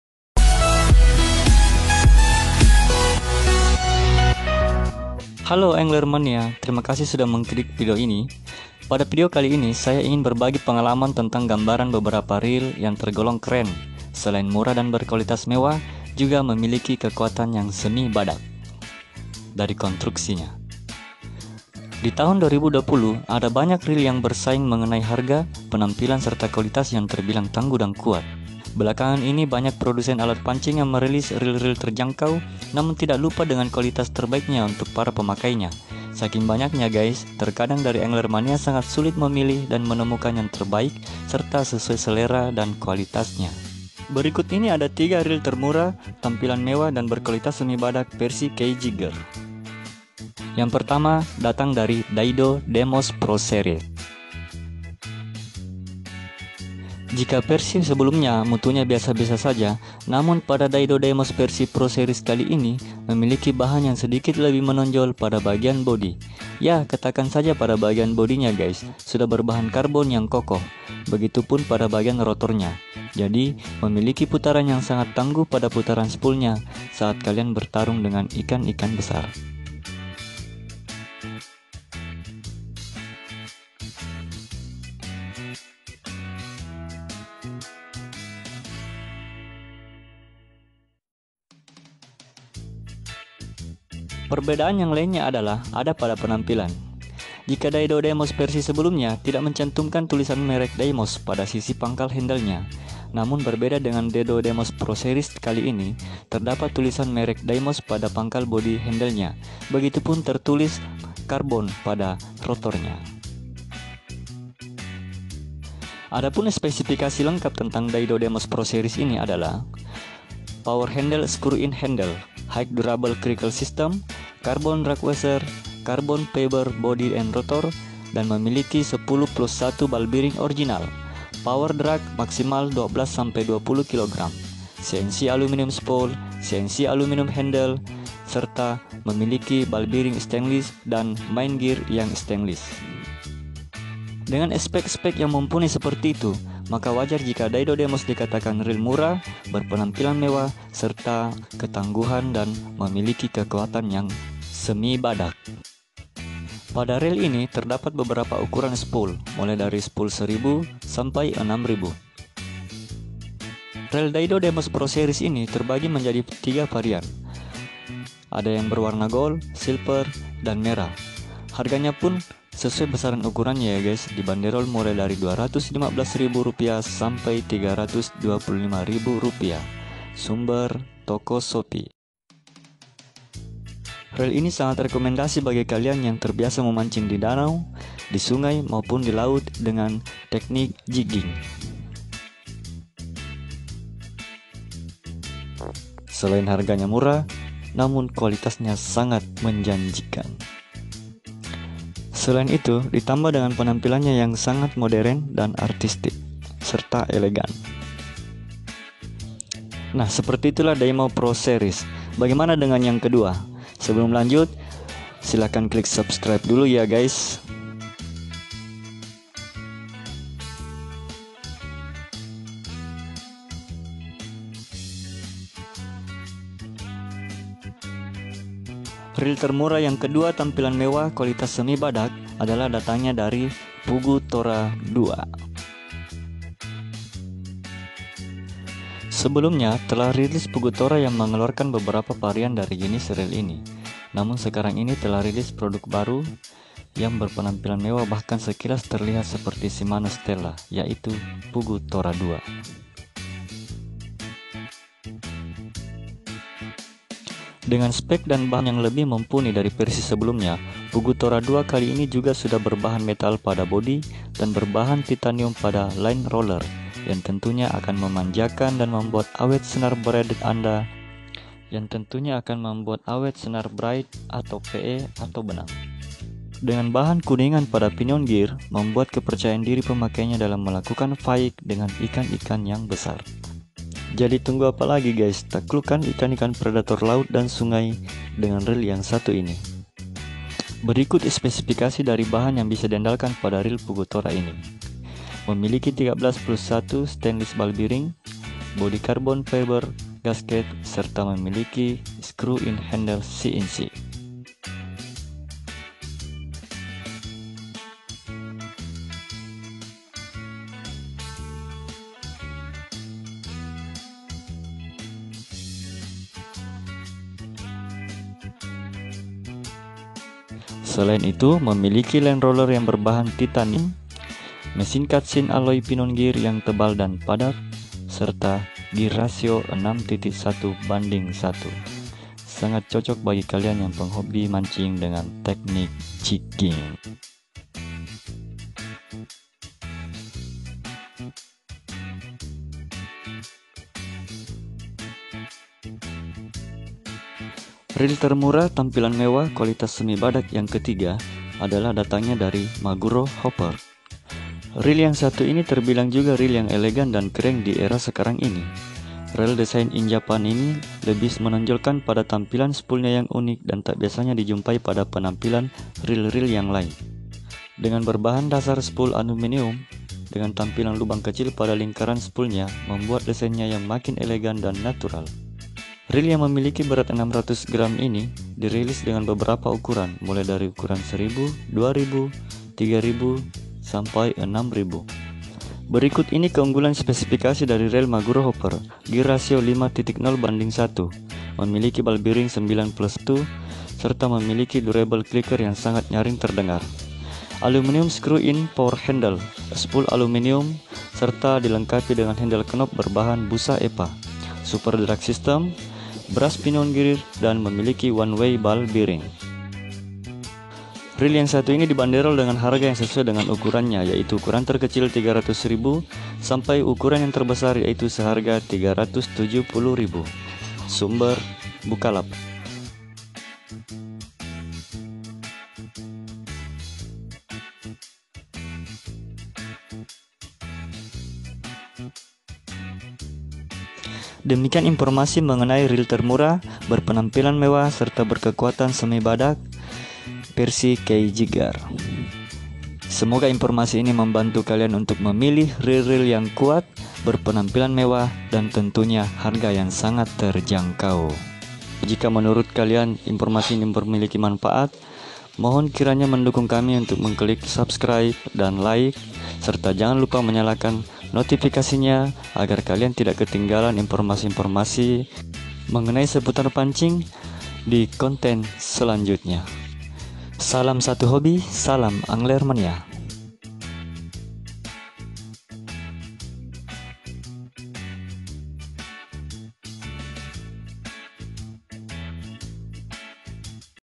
3 reel Pancing termurah terbaik dan berkualitas badak
konten ini berisi gambaran tentang reel pancing murah dan terbaik masa kini 2020 yakni 3 reel pancing termurah terbaik berkualitas badak dan tampilan mewah, yang paling trend dan digemari oleh pemancing mania. Ada daimos proseries, fugutora2, dan Maguro rover.